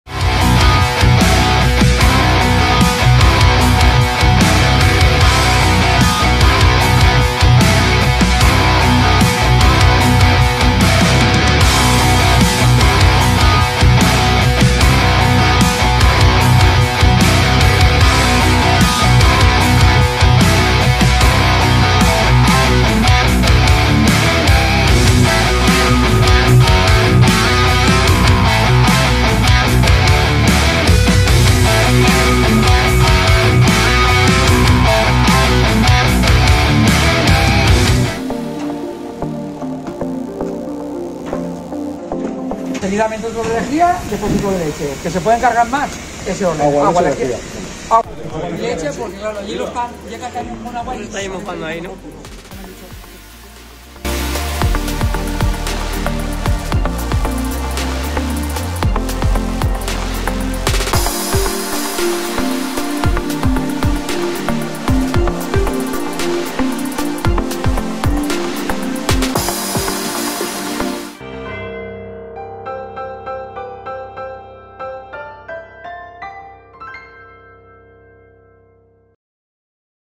Desde entregar agua en Paiporta hasta retirar lodo en centros educativos de Alfafar
"Apoyo incondicional en cada trabajo". Desde entregar agua en Paiporta hasta retirar lodo en centros educativos de Alfafar, los paracaidistas BRIPAC continúan trabajando para apoyar a la población afectada por la DANA
🫵Permaneceremos junto a la población afectada por la DANA hasta superar la emergencia. VamosValencia.